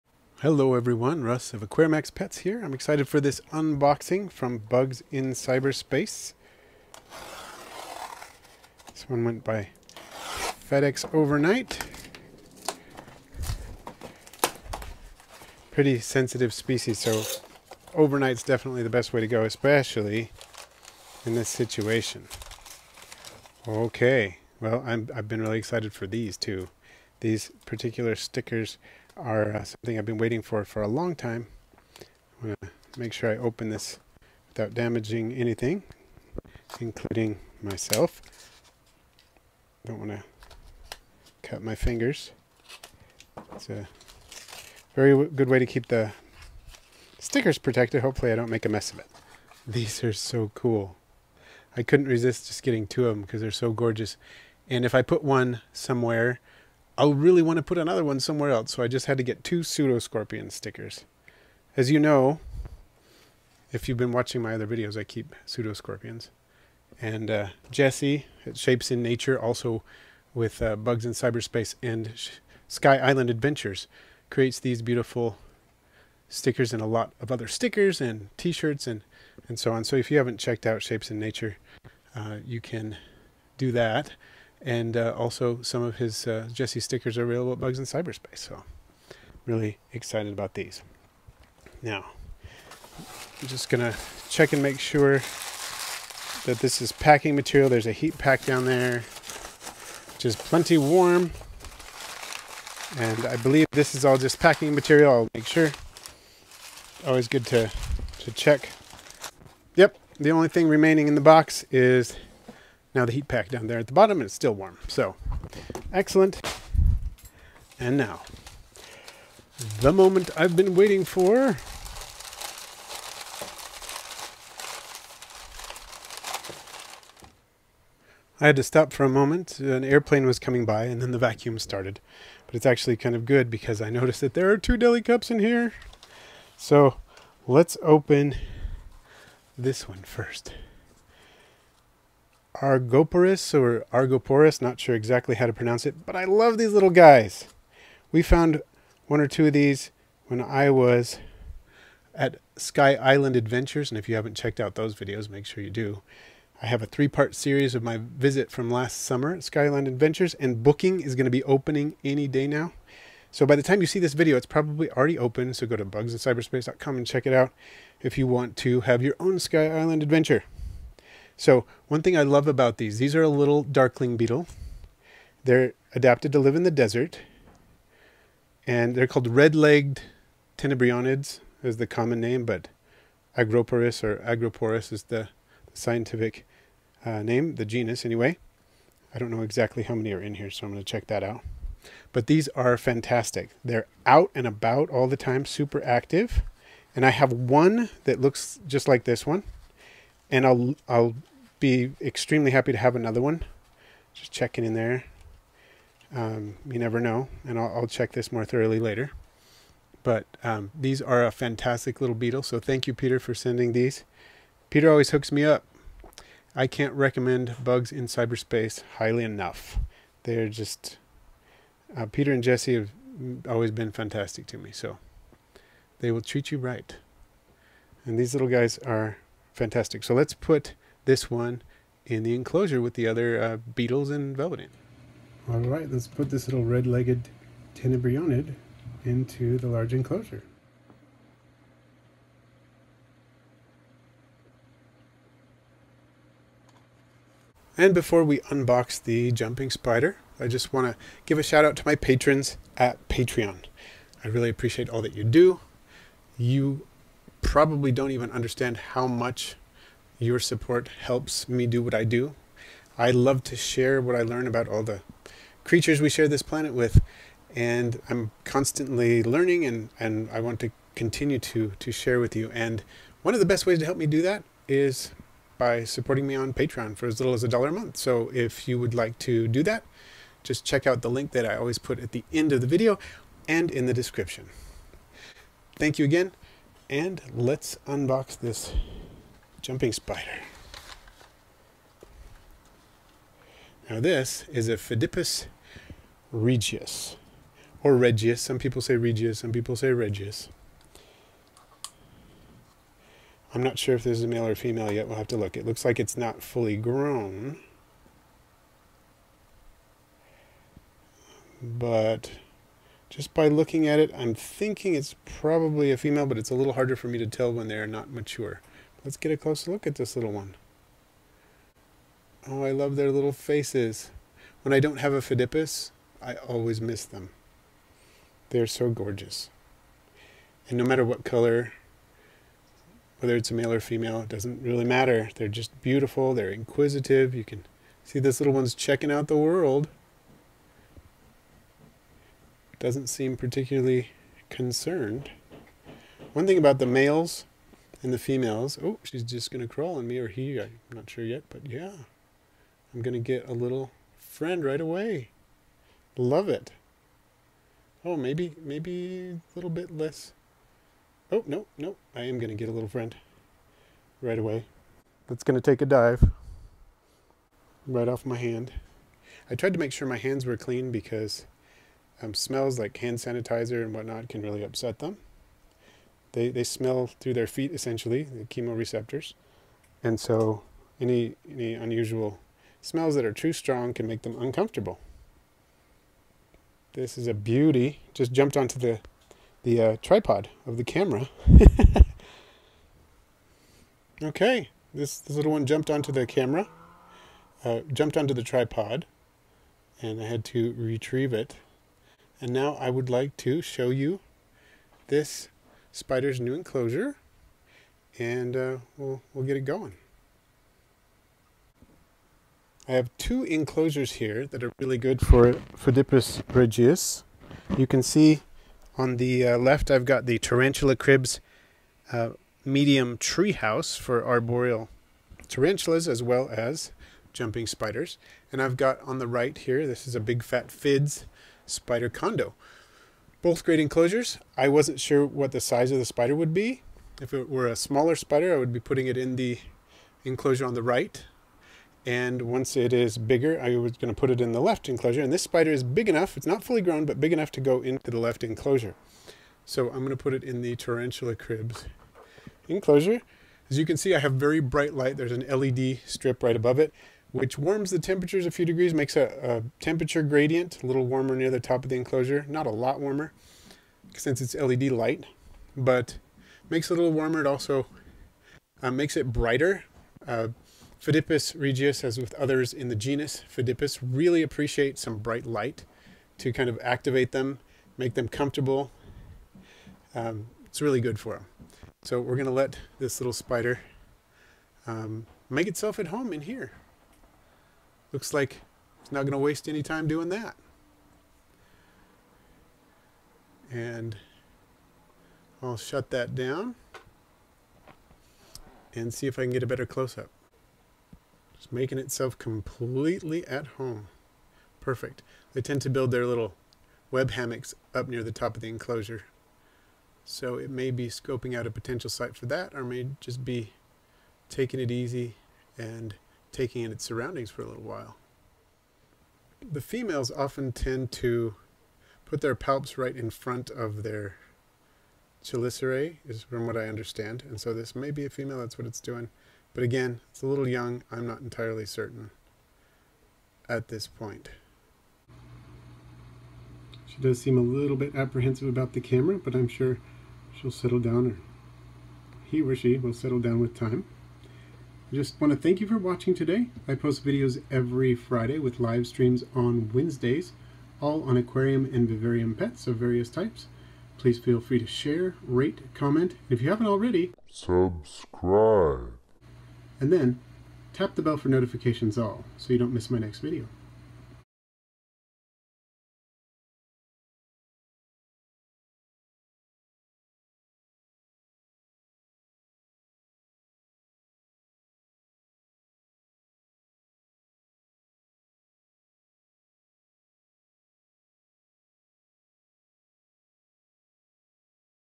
Regal Jumping Spider Unboxing from Bugs in Cyberspace!
Today I unbox a Regal Jumping Spider (Phiddipus regius) I also unbox a surprise invertebrate!

Regal Jumping Spiders are one of the best pet spiders, and best pet invertebrates! They are inquisitive, can learn, and many appear to enjoy interacting with humans. They are easy to care for, and many people who fear spiders find regal jumping spiders cute!